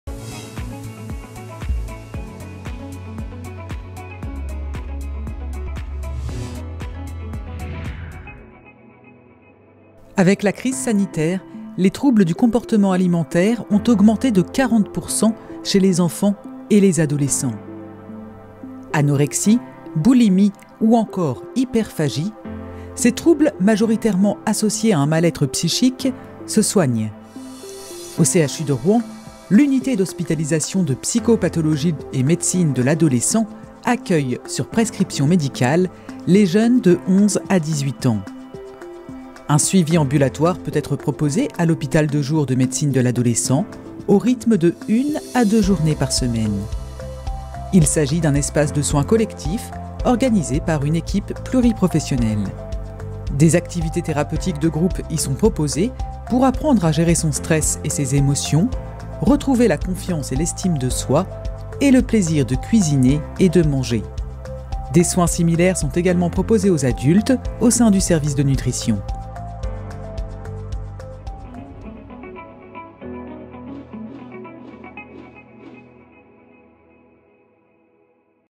Troubles des conduites alimentaires chez l'enfant et l'adolescent
Avec la crise sanitaire, les troubles des conduites alimentaires ont augmenté de 40 % chez les enfants et les adolescents.
Anorexie, boulimie ou encore hyperphagie, ces troubles majoritairement associés à un mal-être psychique, se soignent.
Au CHU de Rouen, l’unité d’hospitalisation de psychopathologie et médecine de l’adolescent accueille, sur prescription médicale, les jeunes de 11 à 18 ans.
Un suivi ambulatoire peut être proposé à l’hôpital de jour de médecine de l’adolescent, au rythme de 1 à 2 jours par semaine. Il s’agit d’un espace de soins collectifs organisés par une équipe pluri professionnelle.
Des activités thérapeutiques de groupe y sont proposées, pour apprendre à gérer son stress et ses émotions, retrouver la confiance et l’estime de soi, et le plaisir de cuisiner et de manger.
Des soins similaires sont également proposés aux adultes, dans une unité dédiée, au sein du service de nutrition.

Coordinatrice : Mathilde LAPOTRE
Journaliste : Cyril GARRIGUES
Cameraman : Christophe DUVAL
Monteur vidéo : Cyril GARRIGUES